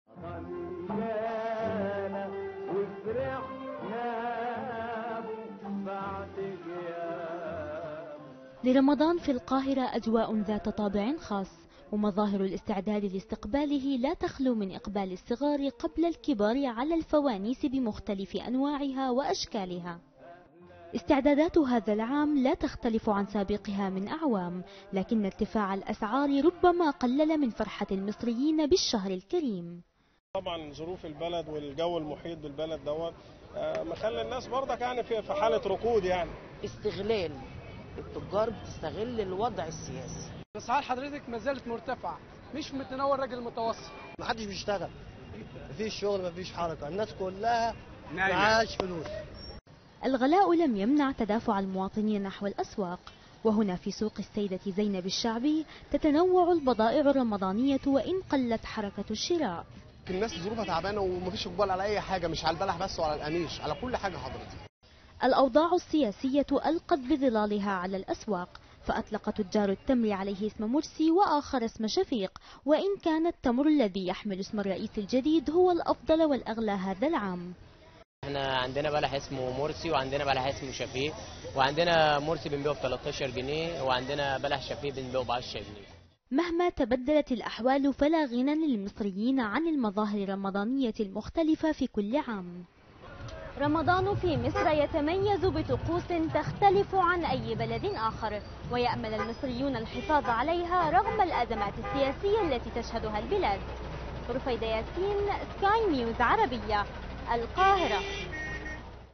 طقوس رمضانية بنكهة مصرية
يستعد المصريون لاستقبال شهر رمضان بطقوس تميزوا بها عبر قرون، ودائما ما تنعكس الأحوال الاقتصادية والاجتماعية على أسواق رمضان، لكن ذلك لم يقلل من فرحتهم بقدوم الشهر الكريم.